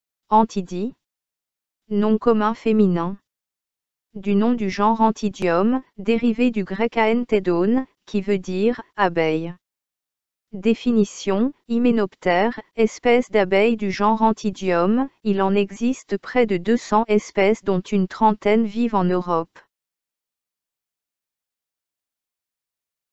Anthidie | Dictionnaire français audio
Définition de mmun féminin.
Du nom du genre Anthidium, dérivé du grec anthêdôn, qui veut dire « abeille ».
Définition : Hyménoptère, espèce d’abeille du genre Anthidium, il en existe près de 200 espèces dont une trentaine vivent en Europe.